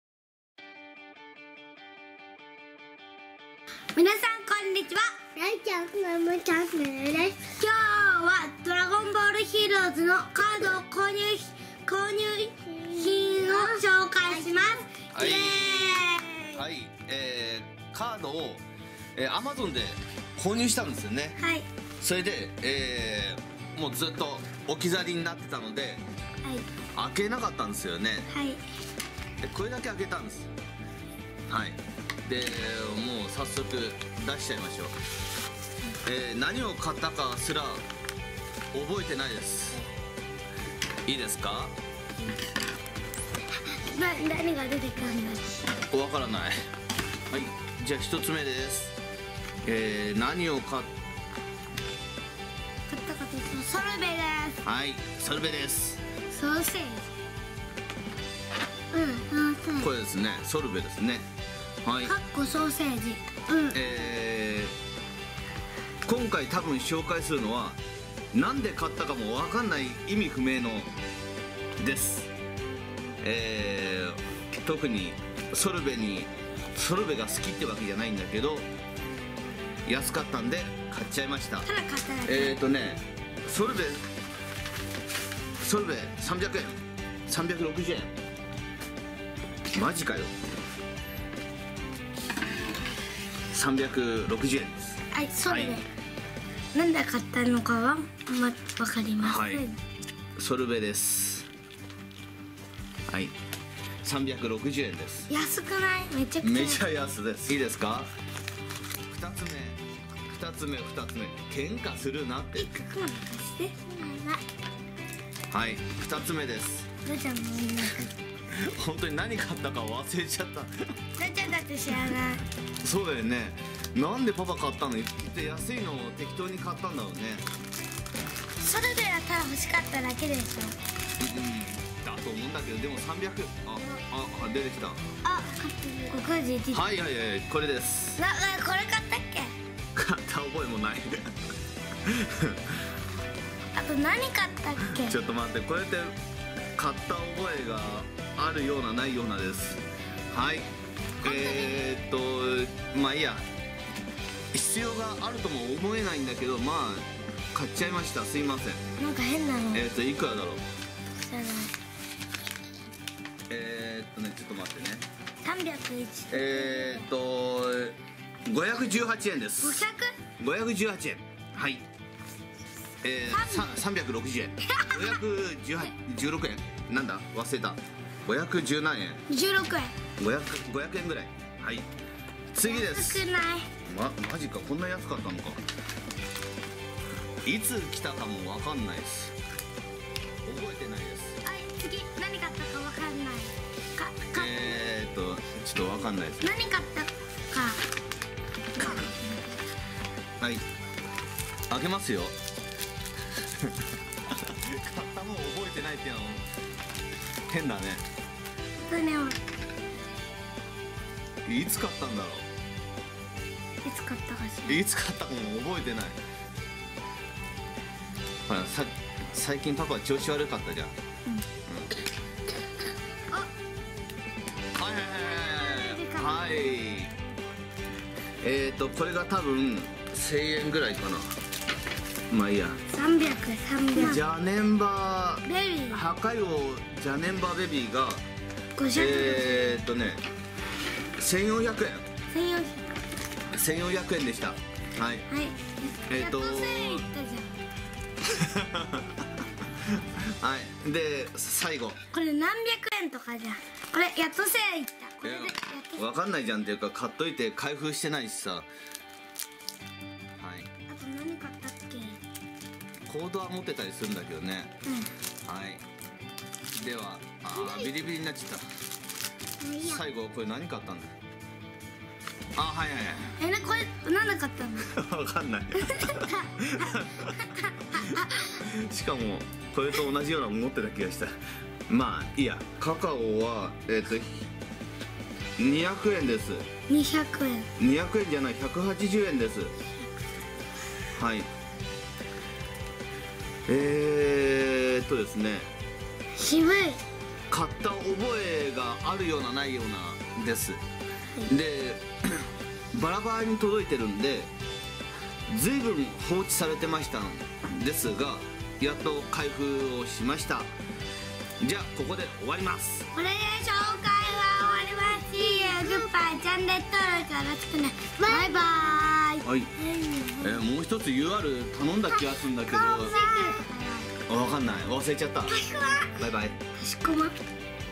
Amazonで購入したのですが2カ月間、開封していないので何が入っているか解りませんww
説明・何で買ったのか不明カードばかりでした・・・？